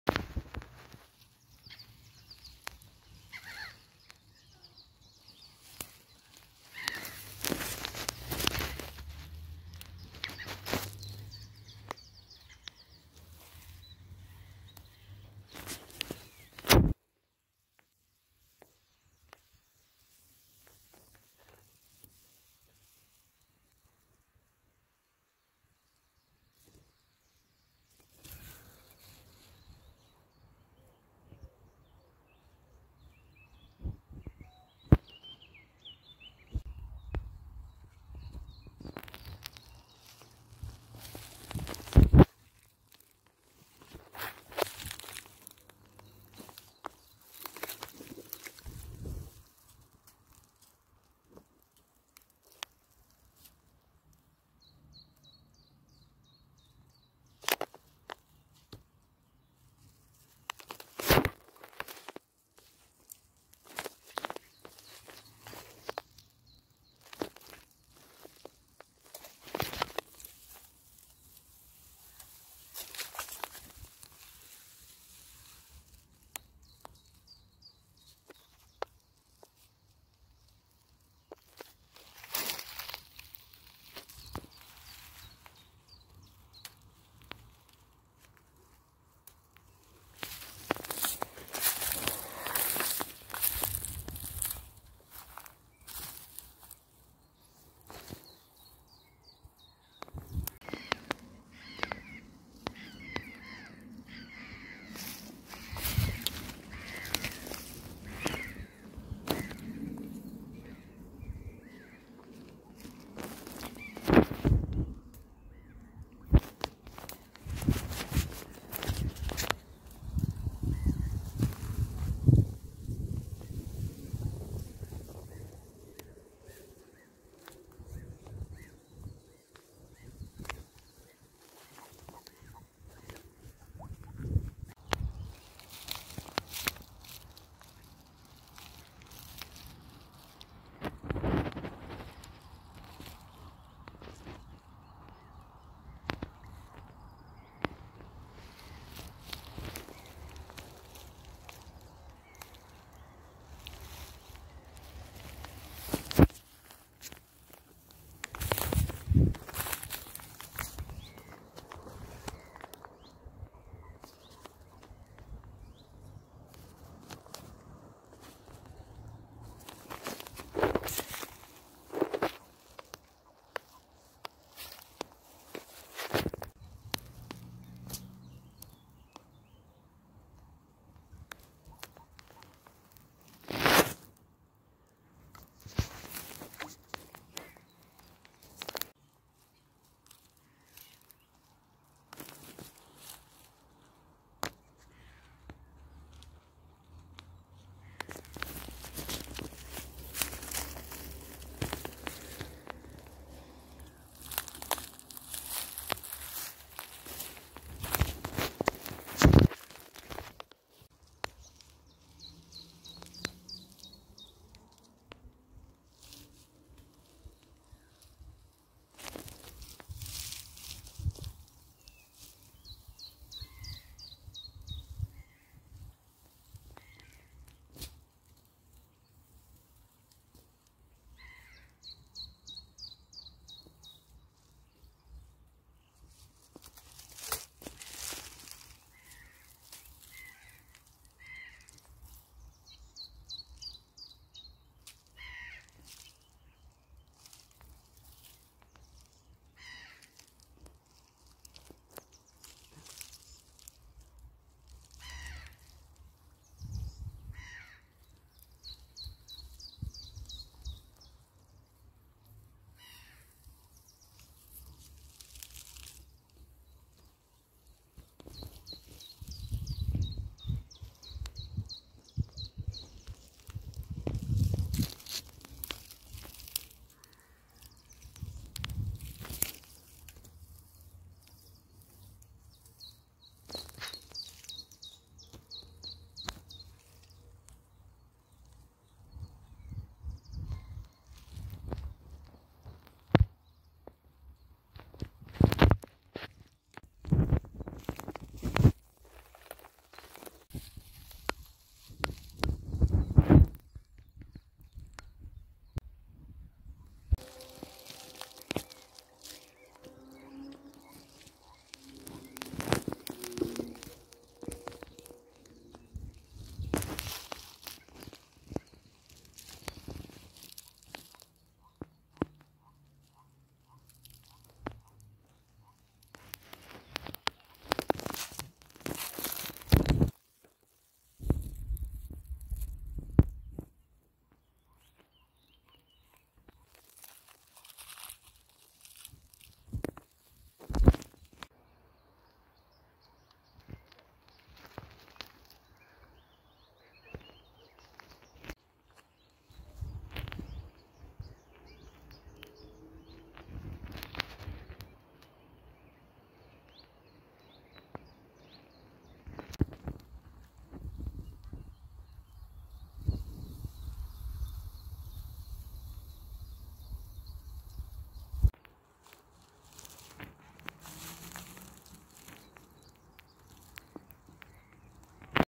Zaskrońce, żaby i ropuchy - wiosenny mix
W dzisiejszym odcinku odwiedziłem pewien staw. Widziałem bobra, zaskrońce, żaby, ropuchy, jastrzębia, pierwiosnki, śmieszki szczupaki, sikorki... taki wiosenny mix. Życzę miłego oglądania :)

Wesołych i zdrowych Świąt!